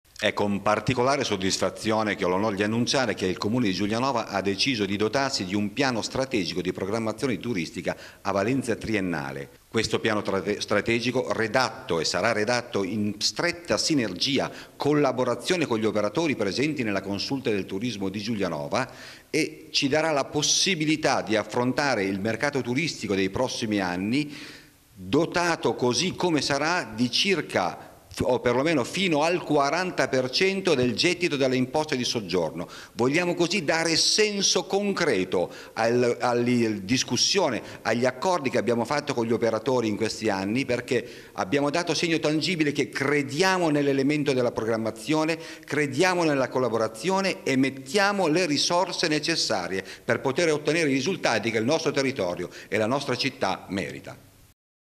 Giulianova. Piano strategico turistico
Il Comune di Giulianova si doterà di un Piano strategico triennale di programmazione turistica che verrà redatto con il coinvolgimento della Consulta del Turismo. L’obiettivo è quello di rendere particolarmente efficace l'intervento pubblico nel settore grazie ai suggerimenti degli operatori